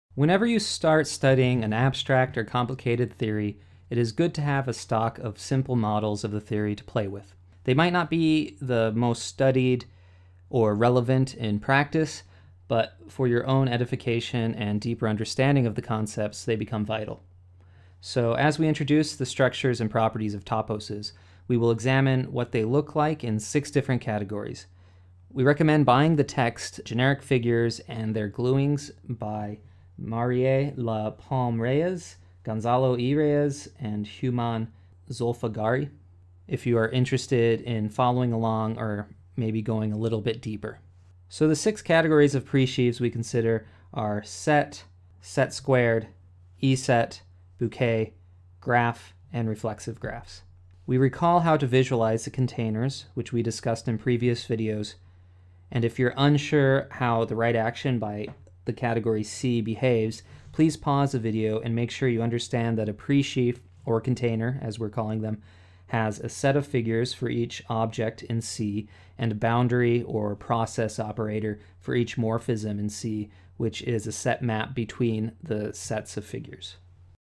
Main Examples of Presheaf Categories
We isolate 6 categories of presheaves we will use to study the structures and properties of toposes in the subsequent videos. Unfortunately, the text "Generic figures and their glueings" is currently out-of-stock A sample of the book is given